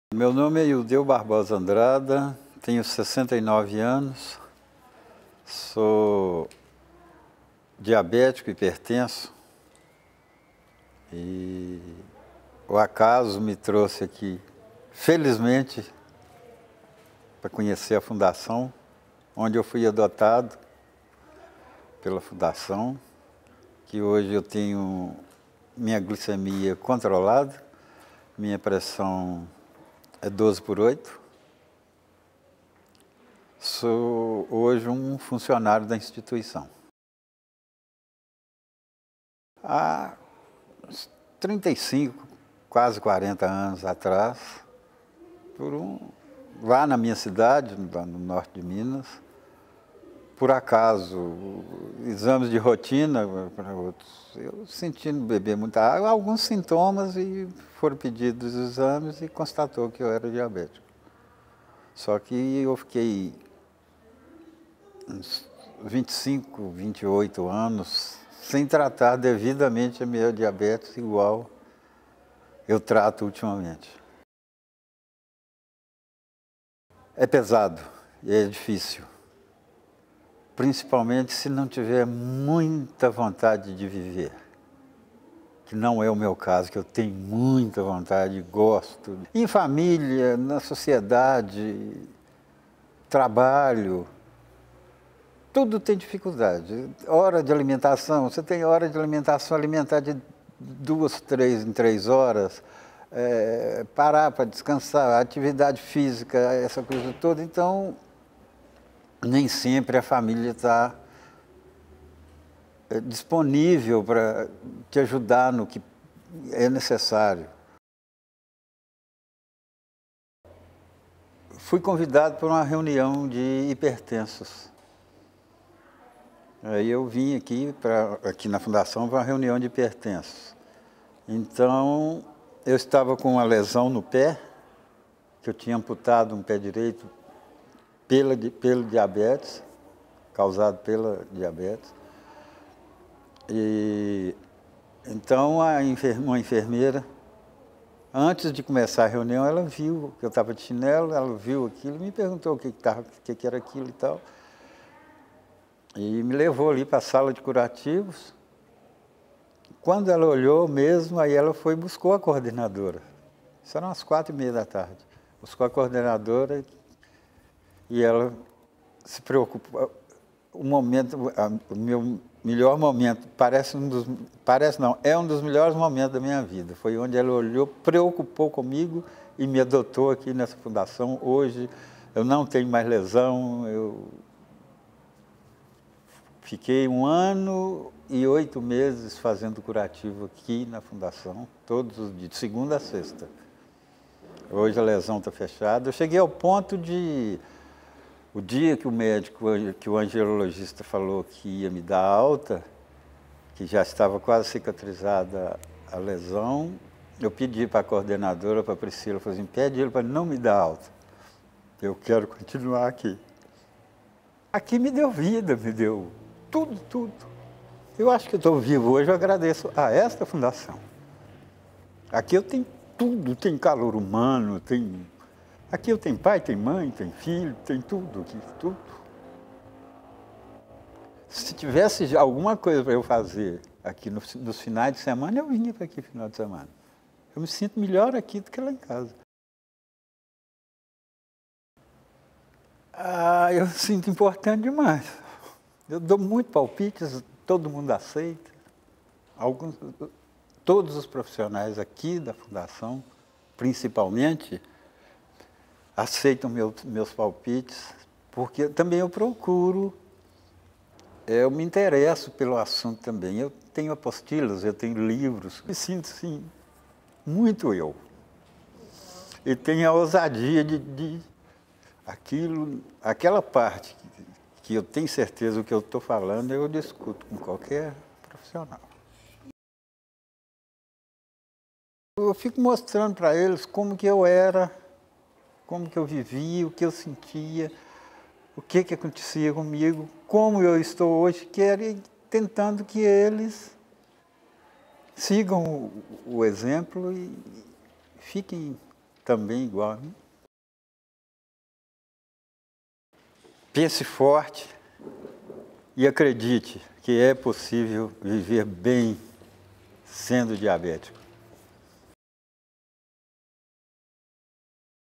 Relato de um diabético beneficiado pelo Modelo de Atenção às Condições Crônicas
Entrevista com Ildeu Andrada – participante do projeto De Volta ao Trabalho